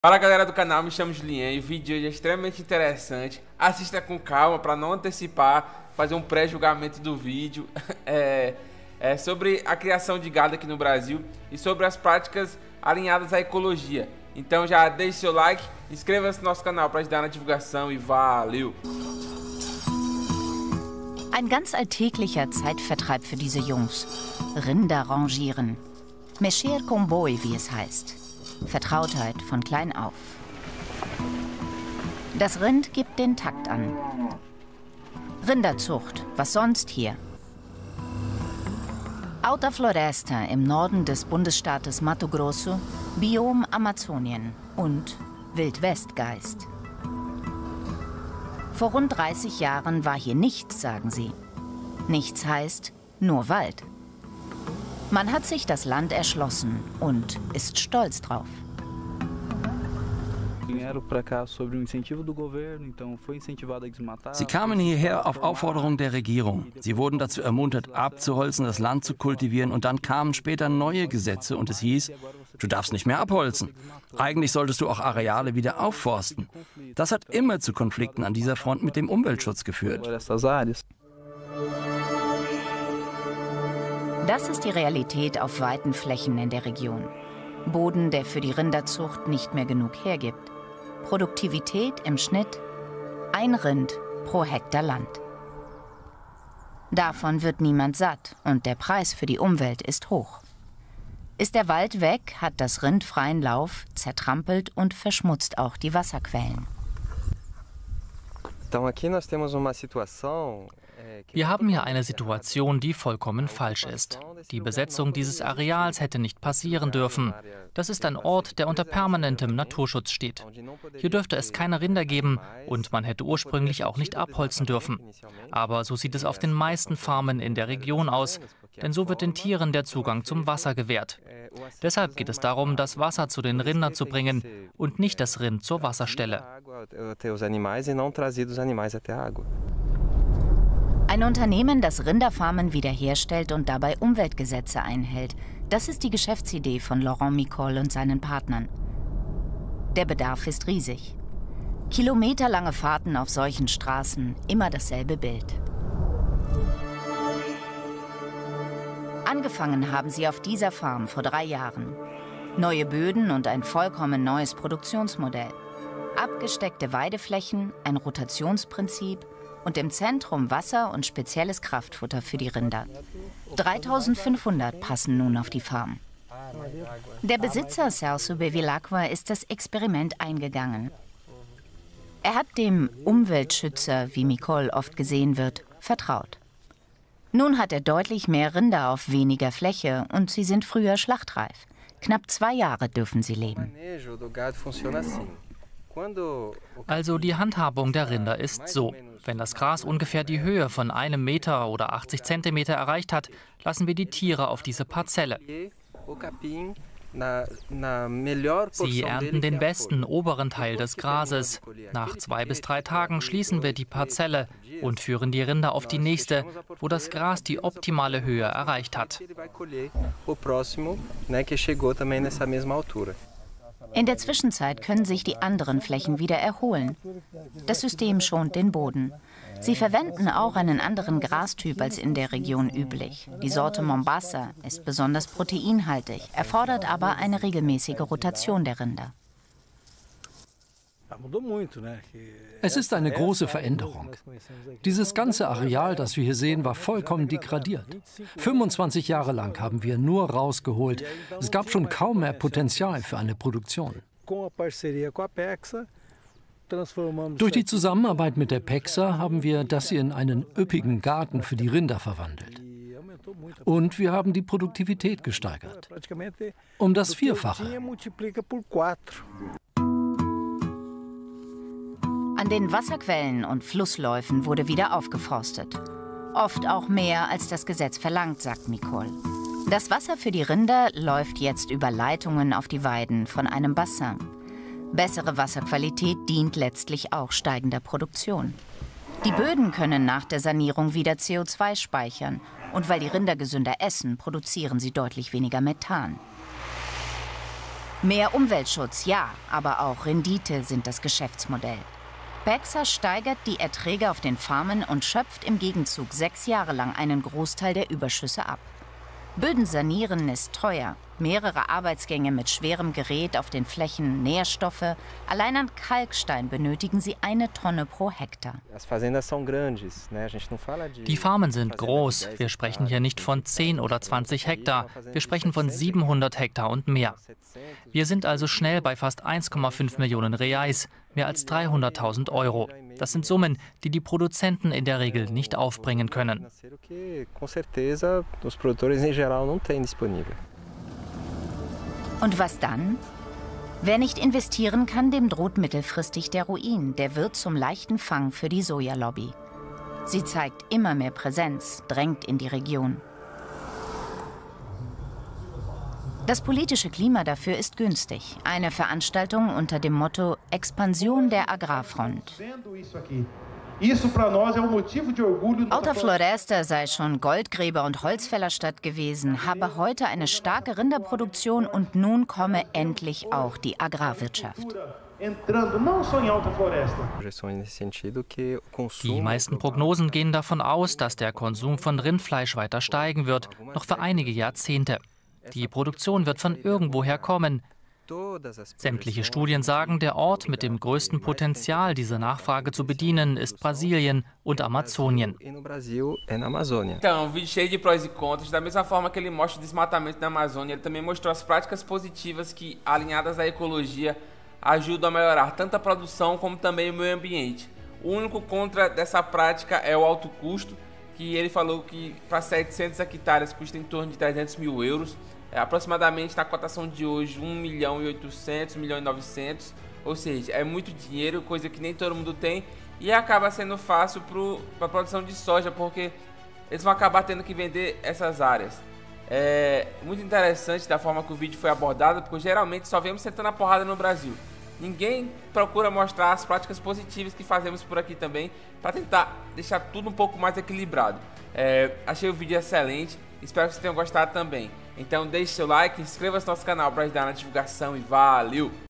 Tv Alemã Mostra O Potencial Do Brasil E Da Criação De Gado Alinhado A Ecologia
O Brasil é um dos maiores produtores de gado do mundo, e quando essas técnicas de criação estão alinhadas a ecologia, viram um exemplo a ser seguido.

Neste vídeo veremos Tv Alemã falando do Brasil e da pecuária brasileira.

Pasto rotativo
Ecologia